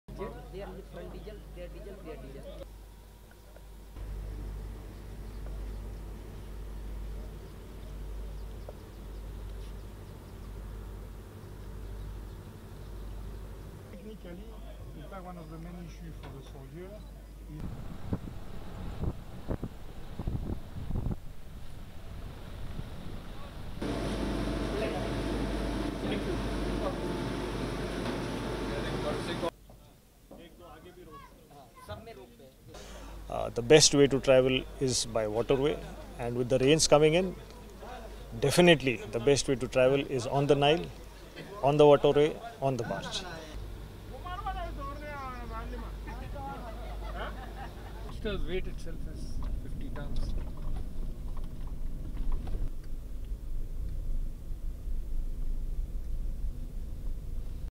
The Nile waterway: A lifeline for UNMISS operations
Meet the Nile Princess…

She plies the lengths, depths and breadths of the River Nile, performing a herculean task of transporting fuel and food rations for UNMISS.

She is a lifeline.

On the eve of a planned departure from Mangala Port, new UNMISS Force Commander drove 80km on a bad dirt road to see what barge transportation entails.

SOUNDBITE (English) United Nations Mission in South Sudan Force Commander, Lieutenant General Shailesh Tinaikar
“The best way to travel is by water way and with the rains coming in definitely the best way to travel is on the Nile on the water-way on the barge.”

It will take her 10 to 12 days to arrive at her destination in Malakal.

Indeed, a better option compared to the unexpectedly long road journeys, which can be compromised due to bad and damaged stretches.